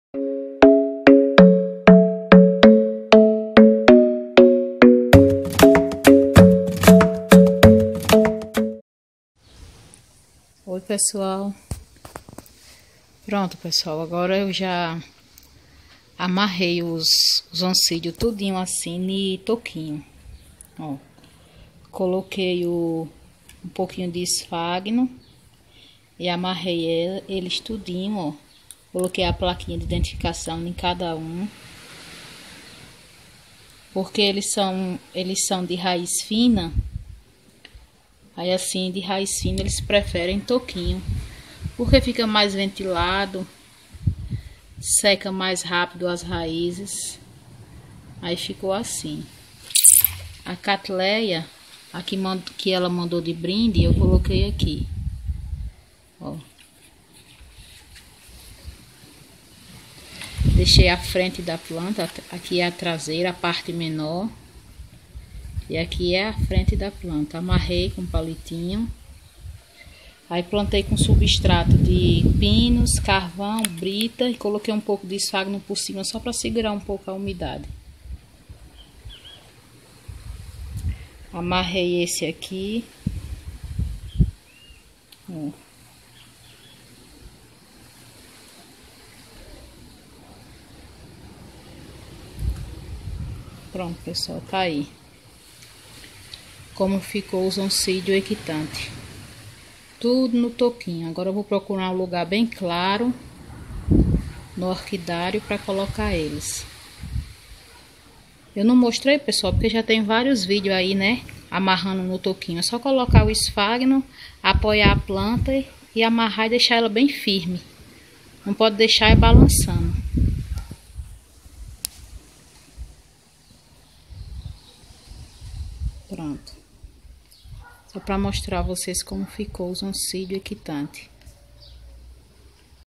Orquídeas em Troncos de Madeira.
Oi pessoal, nesse vídeo mostro como ficou os oncidium equitante em Troncos de madeira, esperando muito que vcs gostem!😘